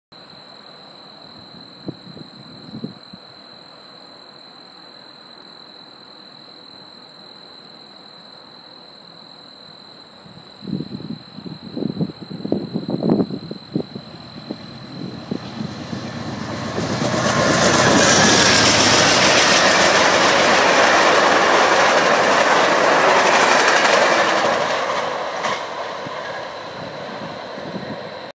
VIA 79 GE P42 905 Bronte GO August 16 2013
Here is VIA 79 with a GE P42 hauling 6 silver cars including a baggage car so that would make it 7 cars passing through Bronte GO station on track 1 with only part of there journey done as they just left Oakville VIA station and are heading towards Aldershot where will likely sit there for 30 minutes before battling the Dundas Subdivision all the way to London ON then onto Windsor ON August 16 2013